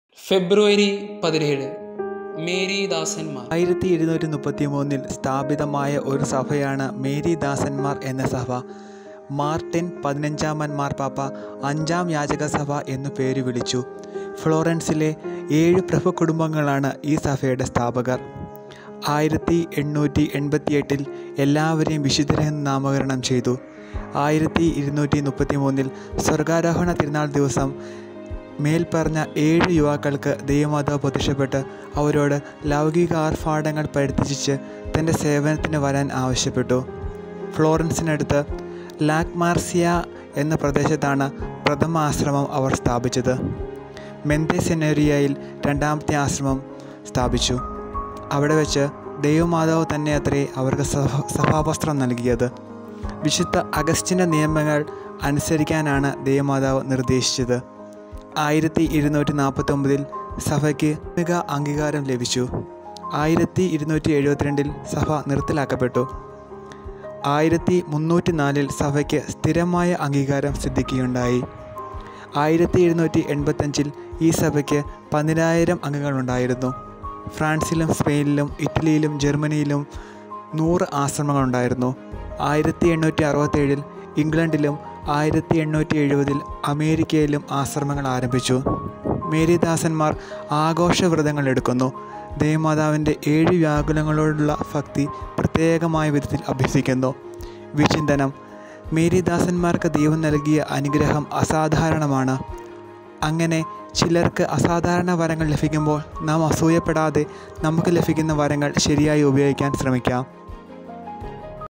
Daily Saints | February-17 | Seven Servite Founders | St.John's minor Seminary, Kothamanagalam
A short Video on Seven Servite Founders
Voice: Br jino
Special Thanks: St.Thomas Delegation MSJ.